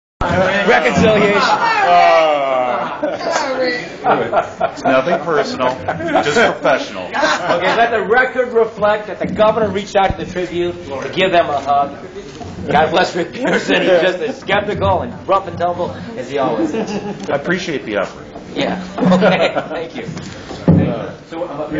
Blagojevich tries to hug Tribune's Rick Pearson
Roving reporter Adam Jadhav turns his video camera on anyone and everyone at the Democratic national convention in Denver.